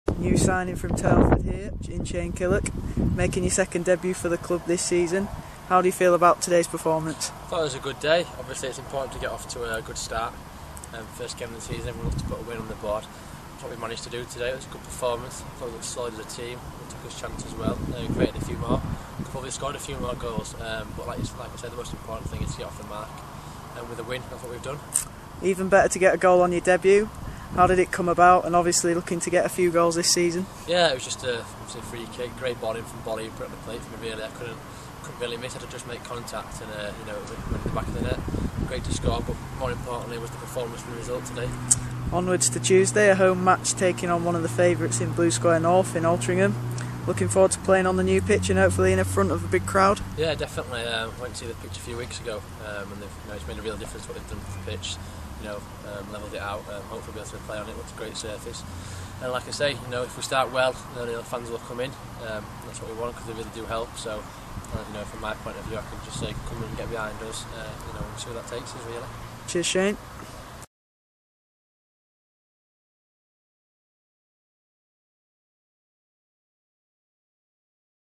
Shane Killock 18th August 2012
Shane Killock talks to Jonny Lally after Harrogate Town beat Hinckley United 2-0 on 18th August 2012.